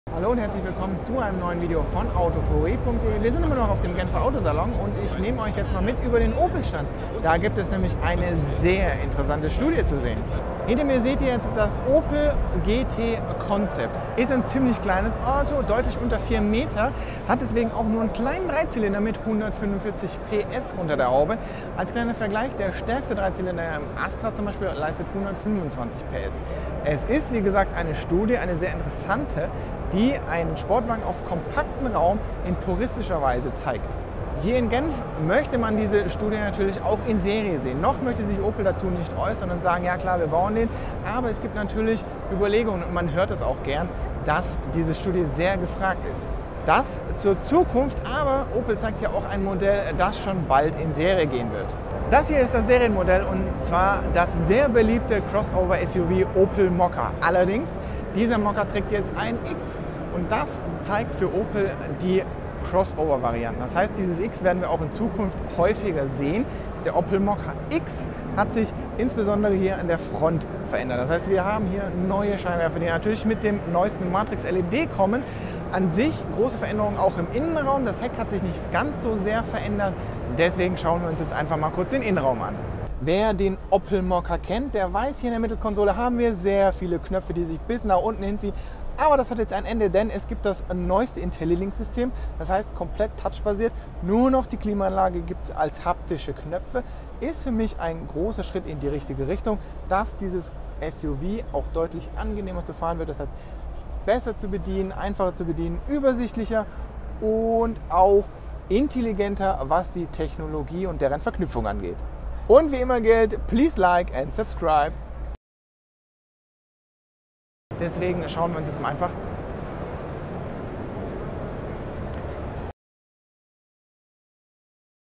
Kurzvorstellung Opel GT Concept und Opel Mokka X
Opel zeigt auf dem 86. Genfer Auto Salon neben einer spannenden Studie auch ein Serienmodell, welches entscheiden verbessert wurde.

Im Fokus steht natürlich die Sportwagen-Studie Opel GT Concept, welche einen Sportwagen auf minimale Größe reduziert und sich auch im Innenraum puristisch darstellt.

Das neue Serienmodell ist der Opel Mokka X, welcher auf den beliebten Mokka folgt. Highlights hierbei sind das optionale Matrix-LED Licht und der neue Innenraum, respektive Mittelkonsole.